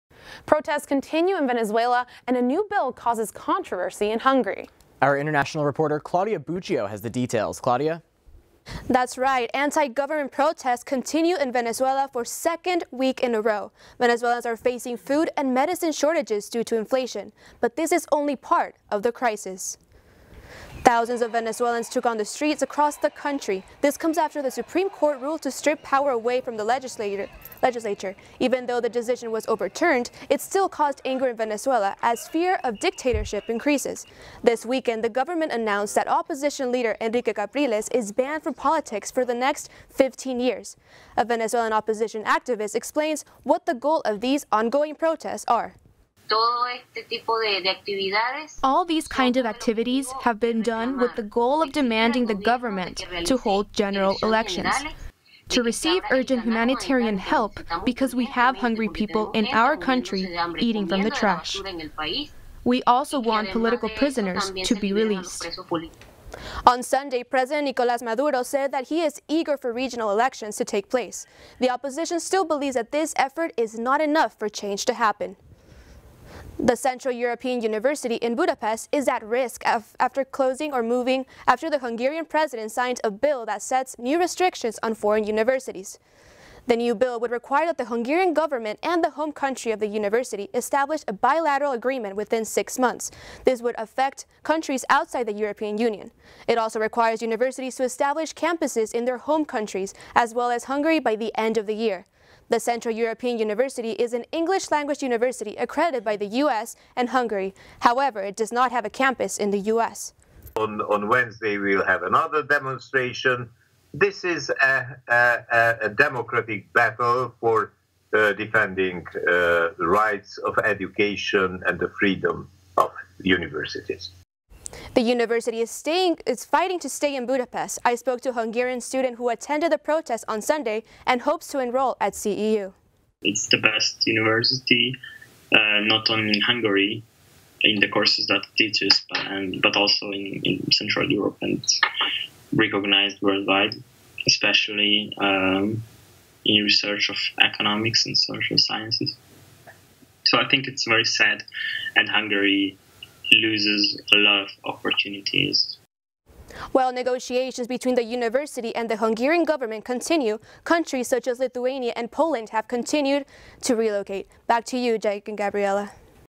Government rulings backfired in Hungary and Venezuela
Protests continue in Venezuela while a higher education bill causes controversy in Hungary